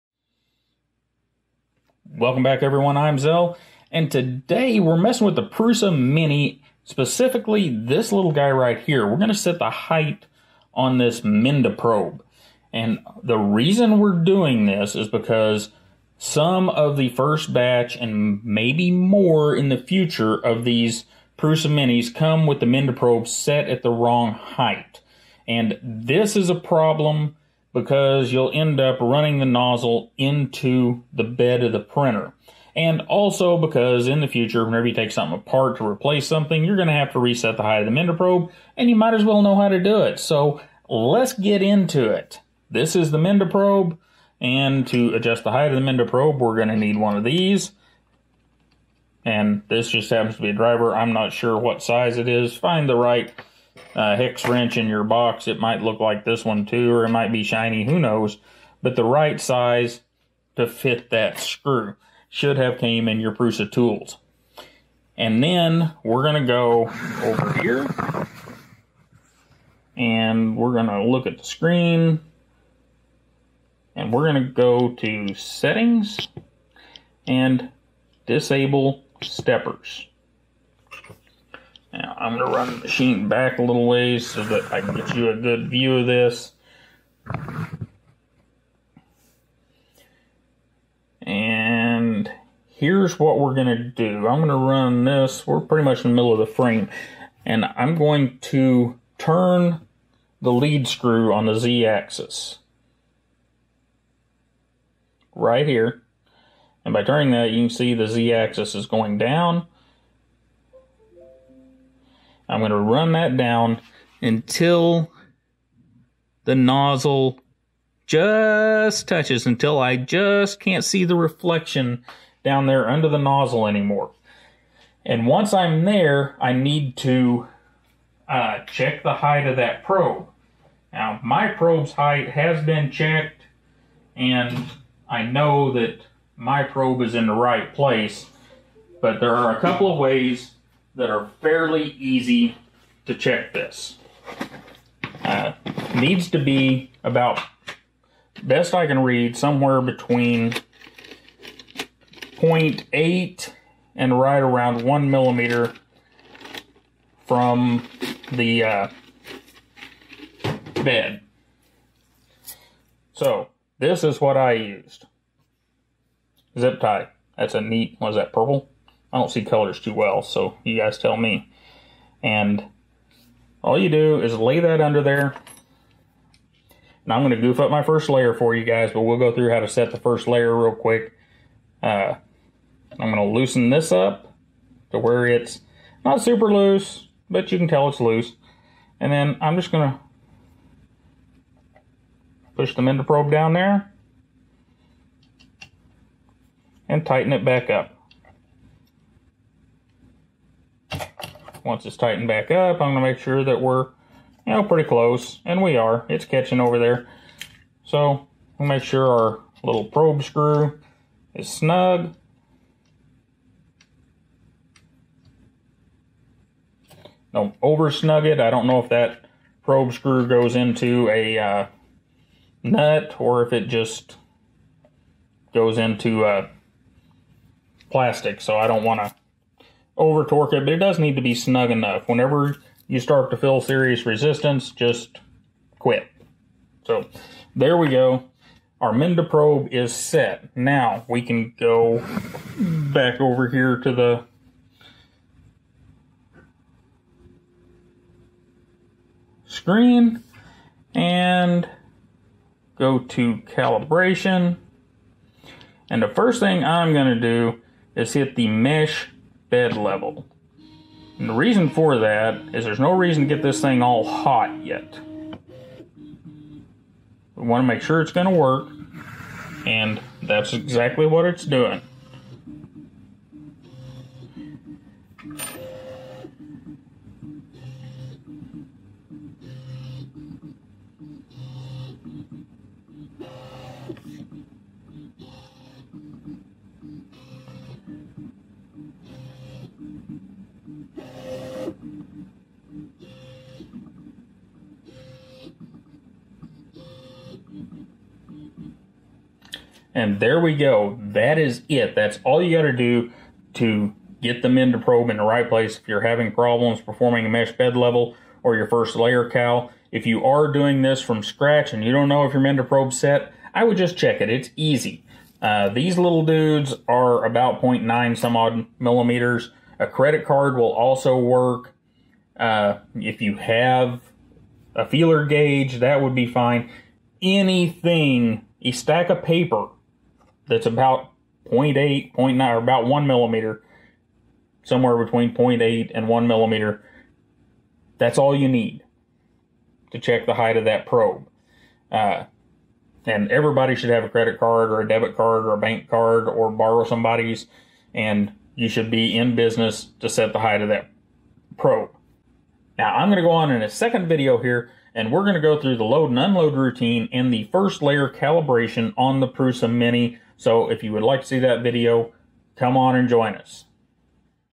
Prusa Mini: Setting The Minda Probe Height
Next up:
Prusa Mini: First Layer Calibration and Filament Loading

The Prusa Mini arrived with the Minda probe not set at the proper height, even though the Prusa manual says it's already set. So, I suggest using this video and the Prusa manual to adjust your Minda probe before you do your first, first layer cal or Mesh bed level. Just to be sure.

Mailing Address:

Zelrick
5517 N. Farmer Branch Rd. #144
Ozark, Mo 65721